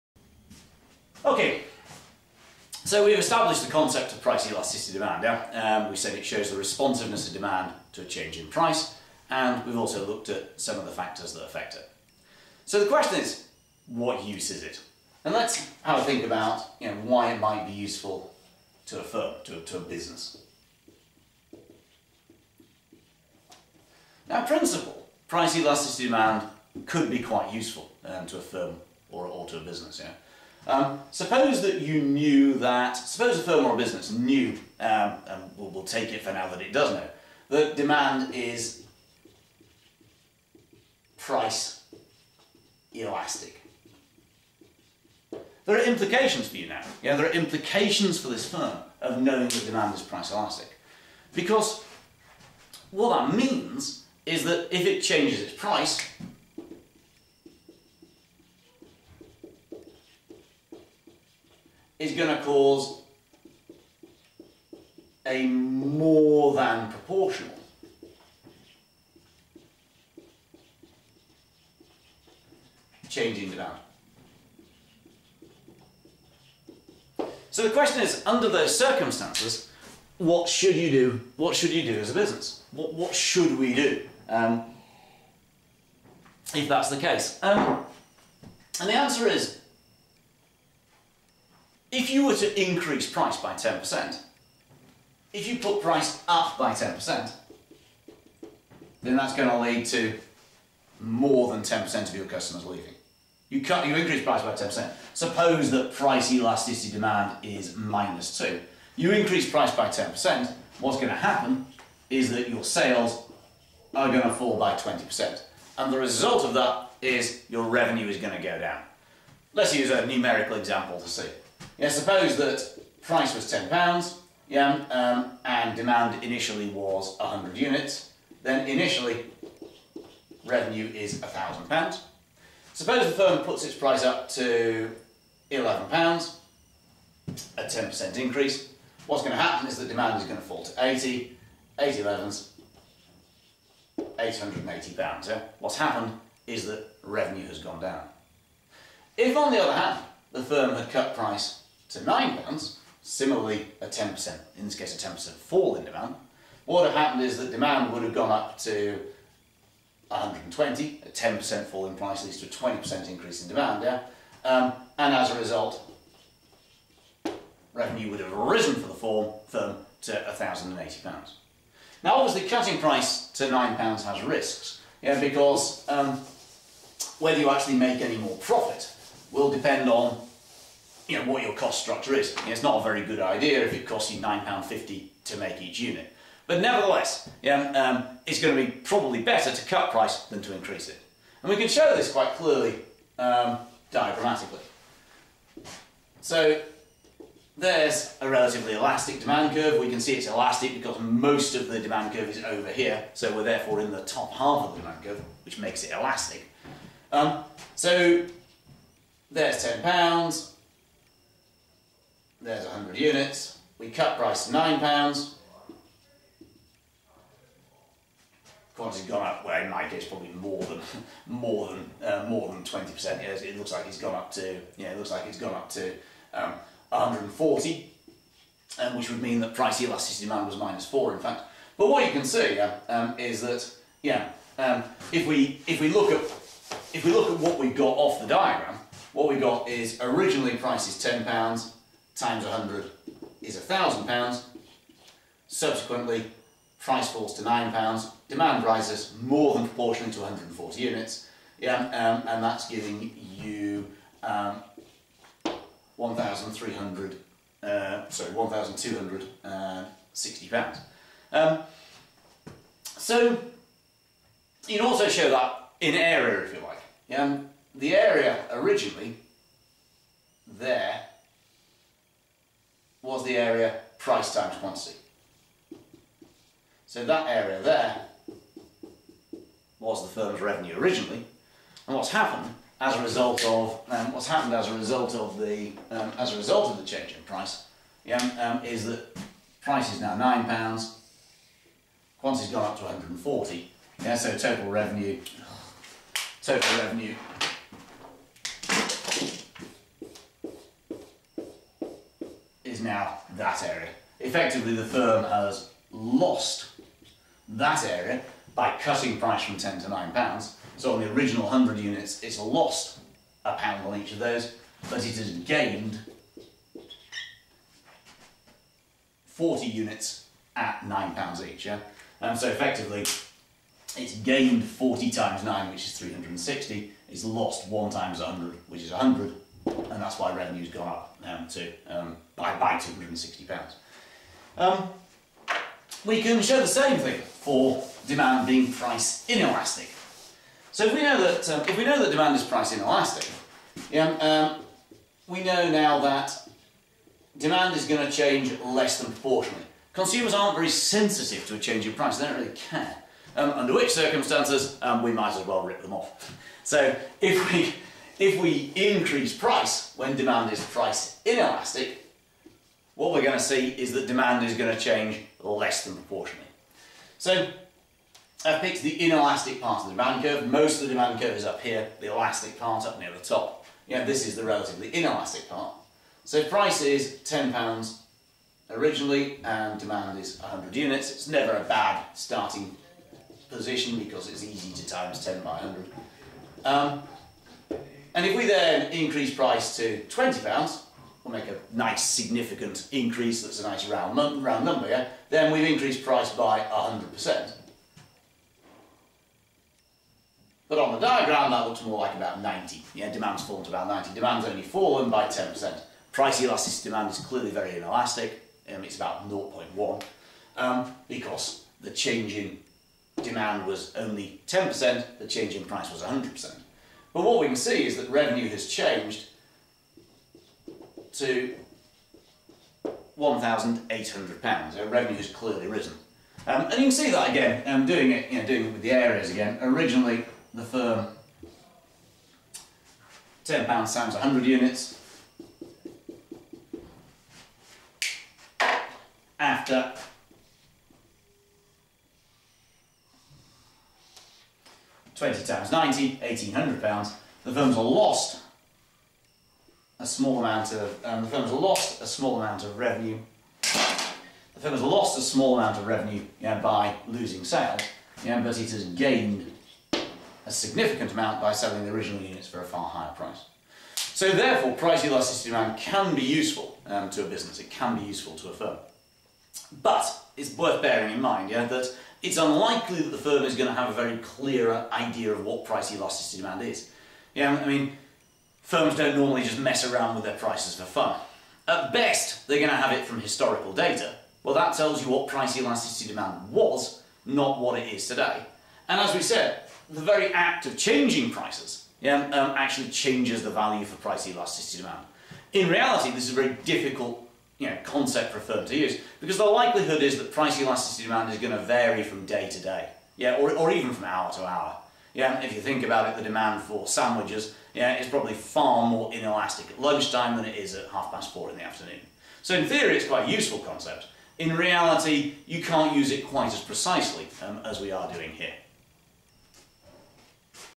Impact of PED on firms' pricing decisions - value of PED to firms
The impact of price elastic and price inelastic demand on firms' decisions